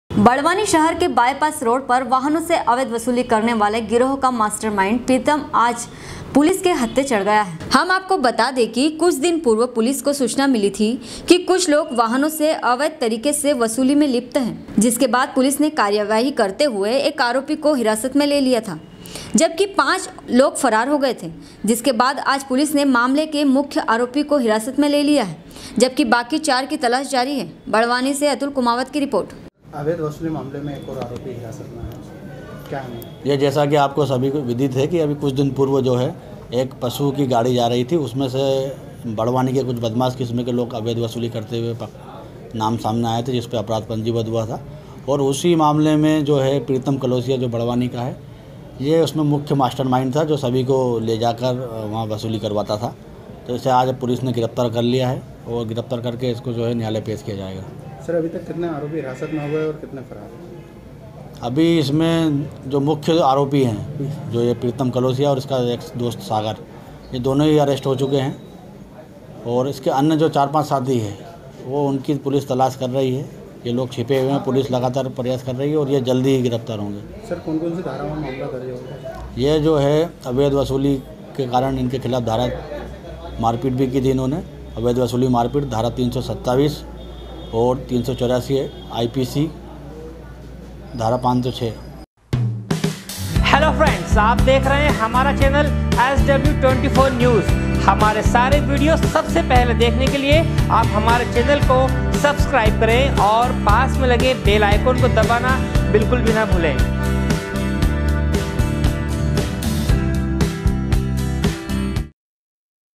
SW 24 News Madhyapradesh
बड़वानी शहर के बायपास रोड़ पर वाहनों से अवैध वसूली करने वाले गिरोह का मास्टमाइंड प्रीतम आज पुलिस के हत्थे चढ़ गया है। बड़वानी से अतुल कुमावत की रिर्पोट